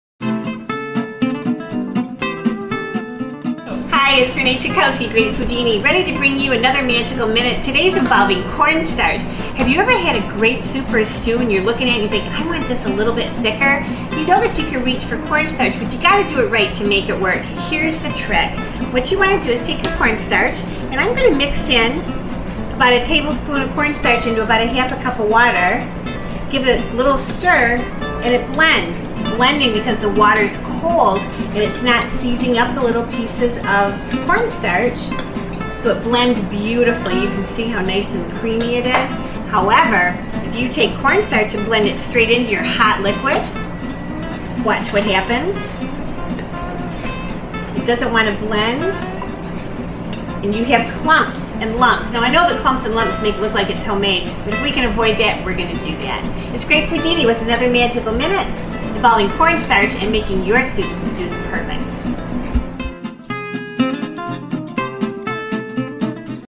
Magical Minute on Making Your Soup Thick!
The Great Foodini shows you how to liven up your soup with corn starch in a way that there will be no clumps!